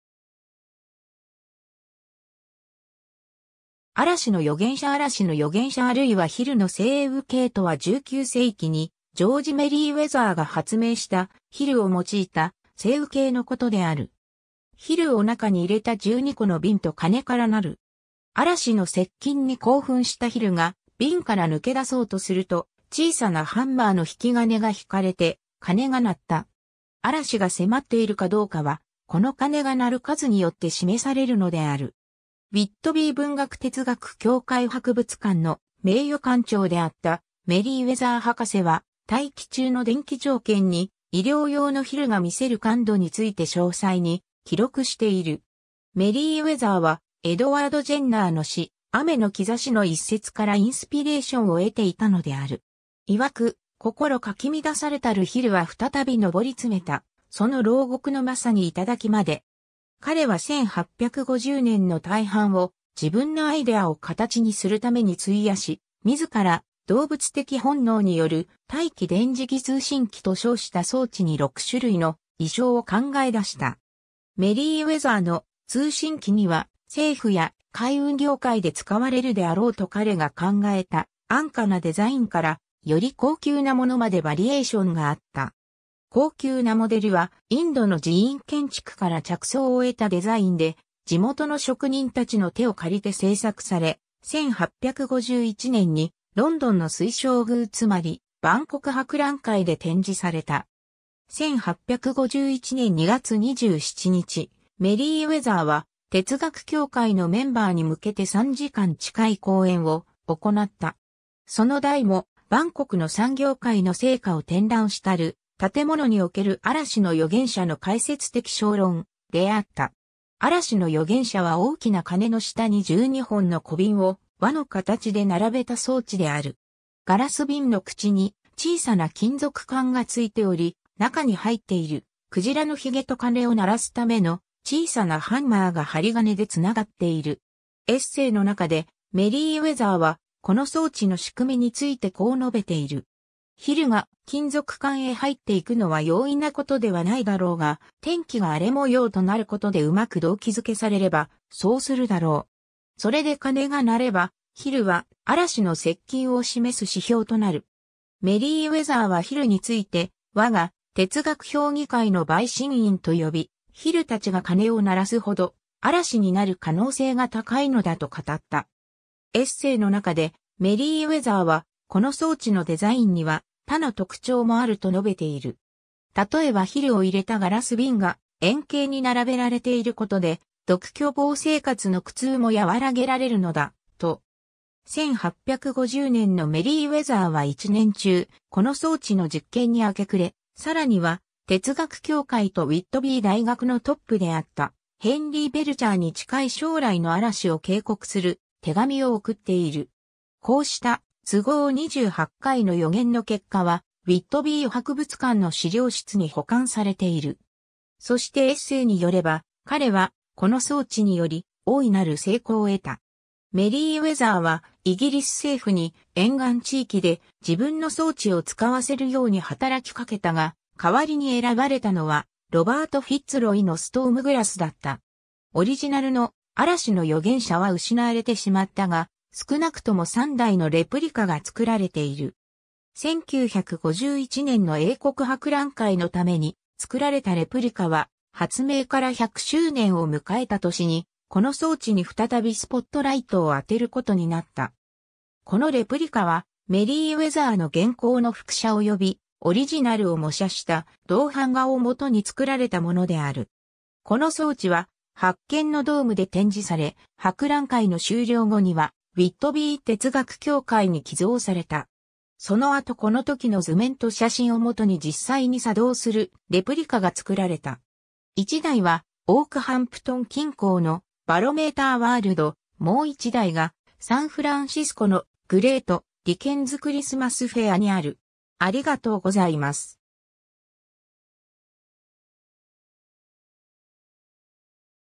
嵐の予言者
気象計測機器・設備
嵐の予言者 嵐の予言者（あらしのよげんしゃ、Tempest Prognosticator）あるいはヒルの晴雨計（ヒルのせいうけい、leech barometer）とは19世紀にジョージ・メリーウェザーが発明した、ヒルを用いた晴雨計のことである。
ヒルを中にいれた12個の瓶と鐘からなる。
嵐の接近に興奮したヒルが瓶から抜け出そうとすると、小さなハンマーの引き金がひかれて鐘が鳴った。
嵐が迫っているかどうかは、この鐘が鳴る数によって示されるのである。
 ウィットビー文学哲学協会博物館の名誉館長であったメリーウェザー博士は、大気中の電気条件に医療用のヒルがみせる感度について詳細に記録している。
メリーウェザーはエドワード・ジェンナーの詩「雨の兆し」の一節からインスピレーションを得ていたのである。
いわく「心かき乱されたるヒルは再びのぼりつめた／その牢獄のまさに頂きまで」。
彼は1850年の大半を自分のアイデアを形にするために費やし、自ら「動物的本能による大気電磁気通信機」と称した装置に6種類の意匠を考え出した。
メリーウェザーの「通信機」には、政府や海運業界で使われるであろうと彼が考えた安価なデザインから、より高級なものまでバリエーションがあった。
高級なモデルはインドの寺院建築から着想を得たデザインで、地元の職人たちの手を借りて製作され、1851年にロンドンの水晶宮つまり万国博覧会で展示された。
 1851年2月27日、メリーウェザーは哲学協会のメンバーに向けて3時間近い講演を行った。
その題も「万国の産業界の成果を展覧したる建物における嵐の予言者の解説的小論」であった。
 嵐の予言者は大きな鐘の下に12本の小瓶を輪の形でならべた装置である。
ガラス瓶の口に小さな金属管がついており、なかにはいっている鯨のヒゲと鐘を鳴らすための小さなハンマーが針金でつながっている。
エッセイのなかでメリーウェザーはこの装置の仕組みについてこう述べている。
 ヒルが金属管へ入っていくのは容易なことではないだろうが、天気が荒れ模様となることでうまく動機付けされればそうするだろう。
それで鐘が鳴れば、ヒルは嵐の接近を示す指標となる。
メリーウェザーはヒルについて「〔我が〕哲学評議会の陪審員」と呼び、ヒルたちが鐘を鳴らすほど、嵐になる可能性が高いのだと語った。
 エッセイのなかでメリーウェザーはこの装置のデザインには他の特徴もあると述べている。
例えばヒルをいれたガラス瓶が円形に並べられていることで「独居房生活の苦痛」も和らげられるのだ、と。
 1850年のメリーウェザーは一年中この装置の実験に明け暮れ、さらには哲学協会とウィットビー大学のトップであったヘンリー・ベルチャーに近い将来の嵐を警告する手紙を送っている.。
こうした都合28回の予言の結果はウィットビー博物館の資料室に保管されている.。
そしてエッセイによれば、彼はこの装置により大いなる成功を得た。
 メリーウェザーはイギリス政府に沿岸地域で自分の装置を使わせるように働きかけたが、代わりに選ばれたのはロバート・フィッツロイのストームグラスだった。
 オリジナルの「嵐の予言者」は失われてしまったが、少なくとも3台のレプリカがつくられている。
1951年の英国博覧会のためにつくられたレプリカは、発明から100周年を迎えた年にこの装置に再びスポットライトを当てることになった。
このレプリカはメリーウェザーの原稿の複写およびオリジナルを模写した銅版画をもとにつくられたものである。
この装置は「発見のドーム」で展示され、博覧会の終了後にはウィットビー哲学協会に寄贈された。
その後このときの図面と写真をもとに実際に作動するレプリカがつくられた。
1台はオークハンプトン近郊の「バロメーター・ワールド」、もう1台がサンフランシスコの「グレート・ディケンズ・クリスマス・フェア」にある。